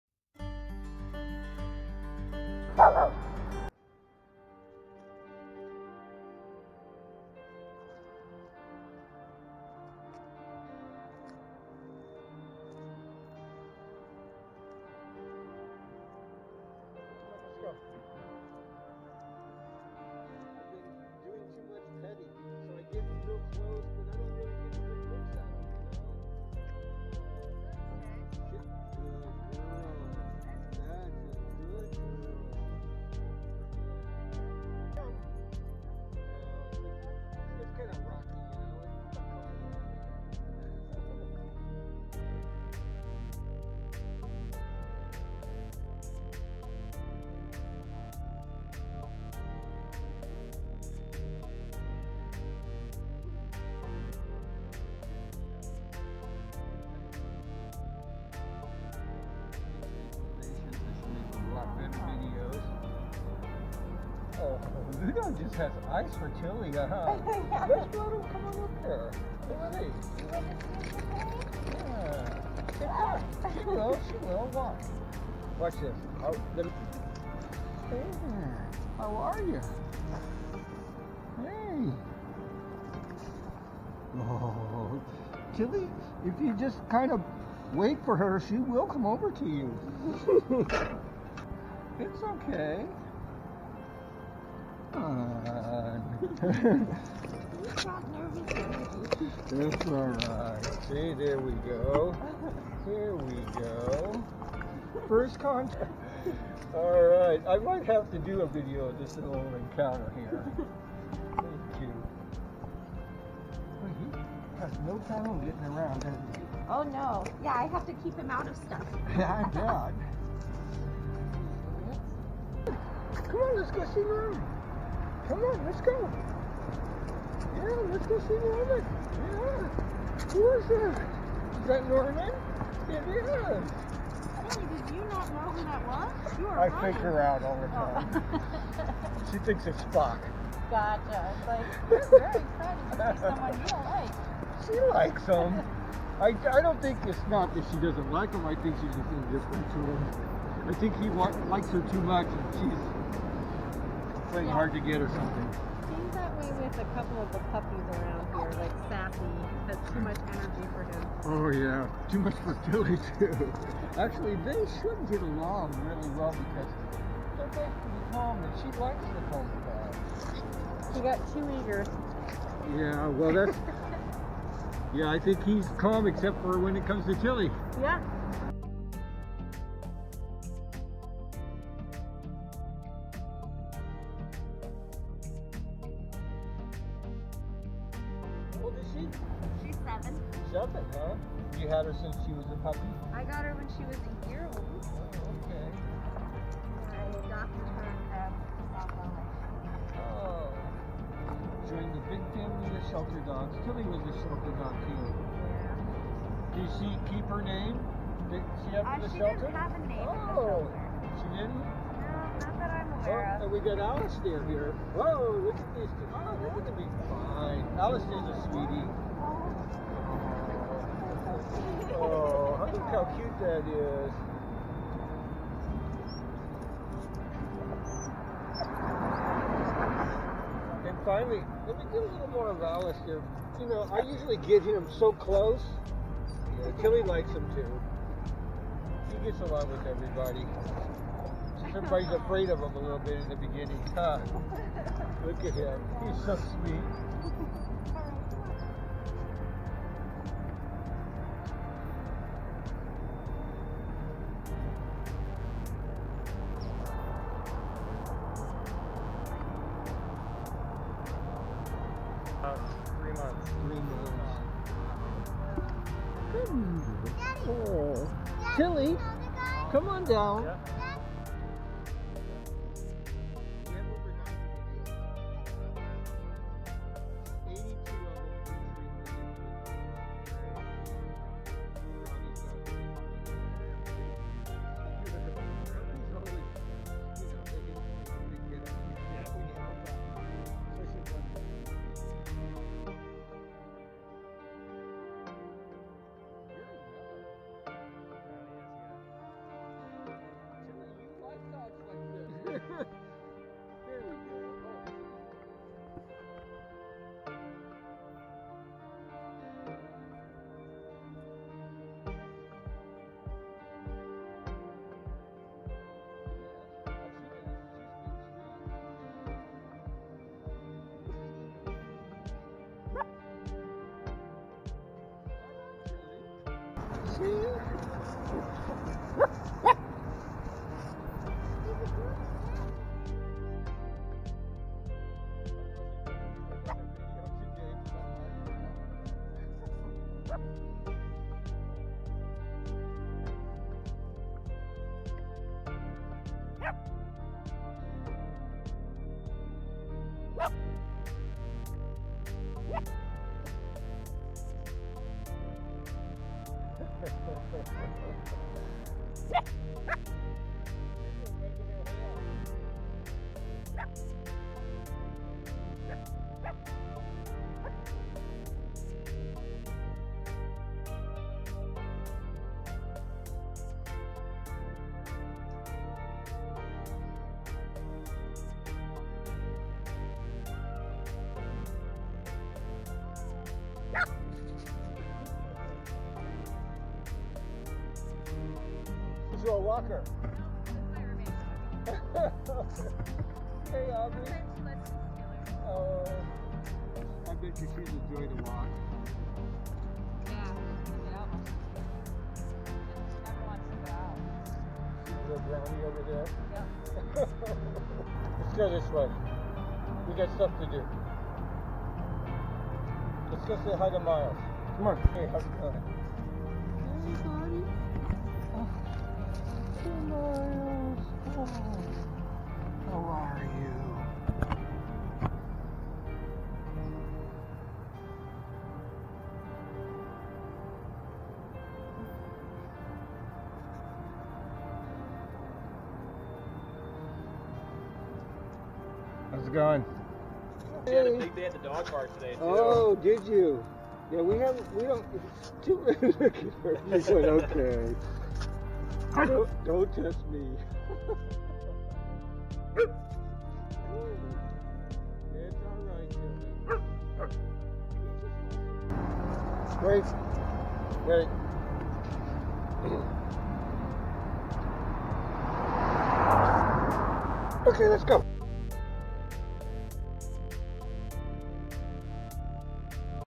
Dog Walk Meetups #4 A 9 Dog Day (In Tilly Speak)
In this video, Lilly, Ludo, Norman, Sienna, Alistair, Puppy Brownie!, Aubrey, Miles and Shadow.  Kind of an overcast day, but the dogs were shining bright.  How is it possible for me to have such fun on these walks.  Brownie was a special treat.  To have a precocious puppy bouncing everywhere, the cuteness factor goes off the chart.
No stress here, on Tilly TV